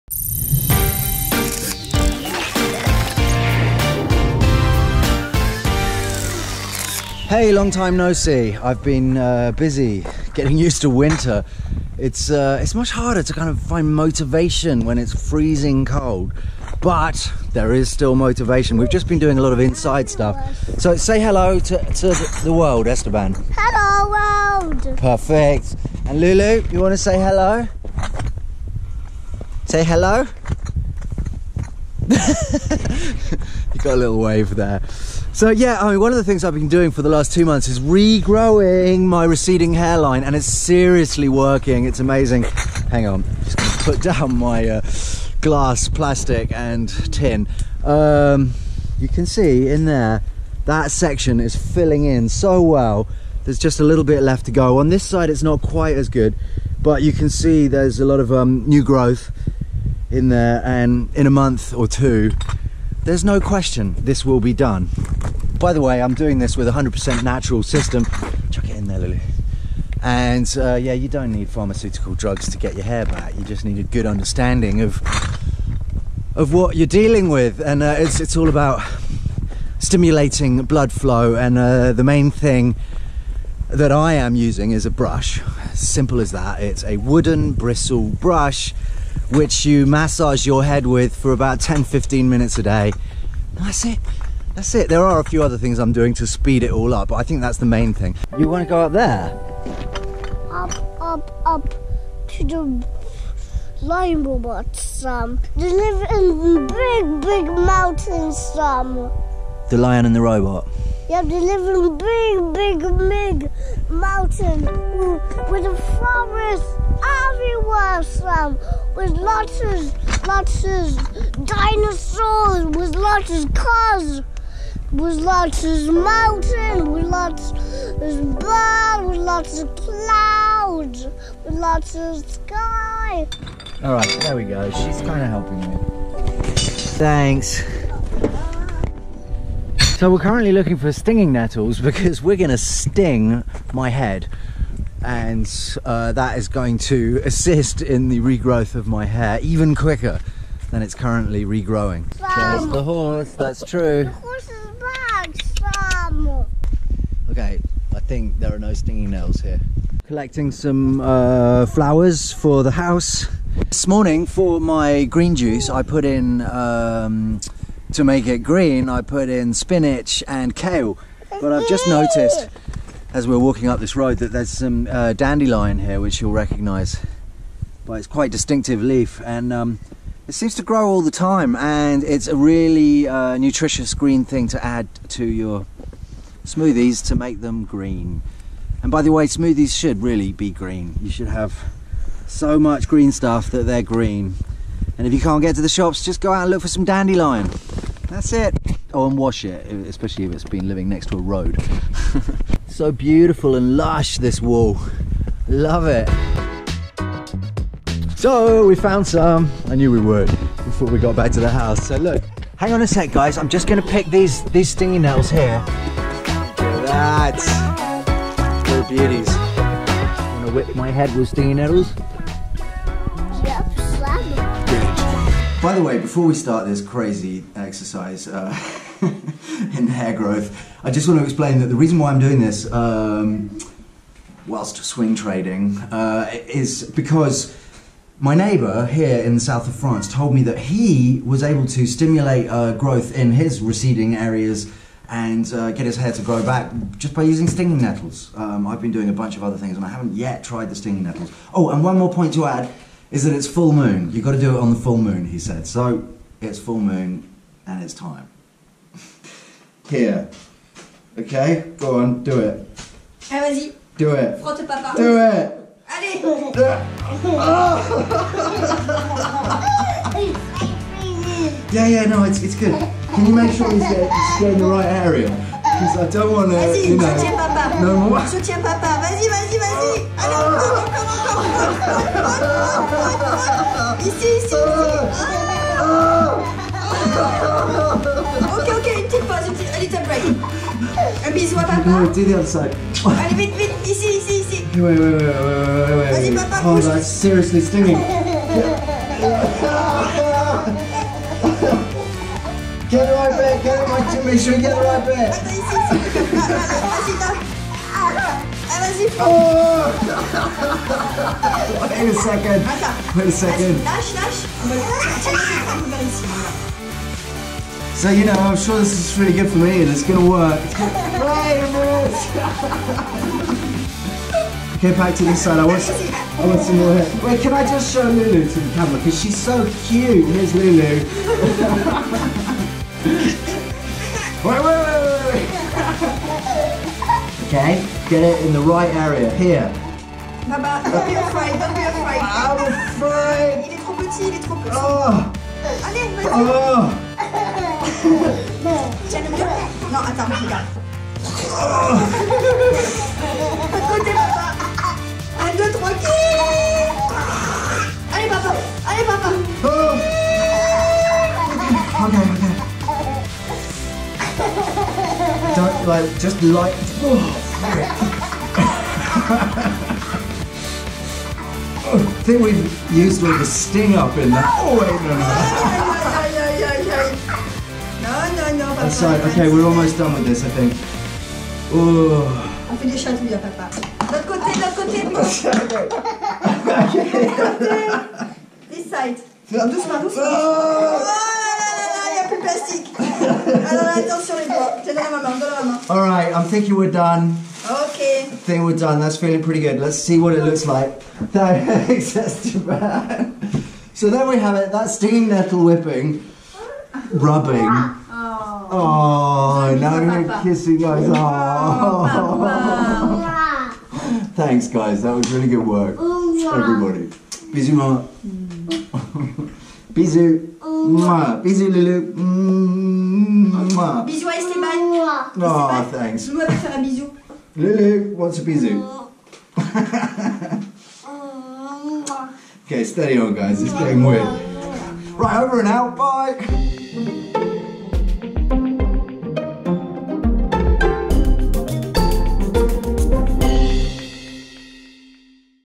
Stinging my head with nettles to encourage hair growth!
As mad as this sounds it makes a lot of sense given that enhanced blood flow encourages those dormant follicles to come back to life.  Almost six hours after this 'stinging nettle whipping' I could still feel the blood pumping through the desired areas.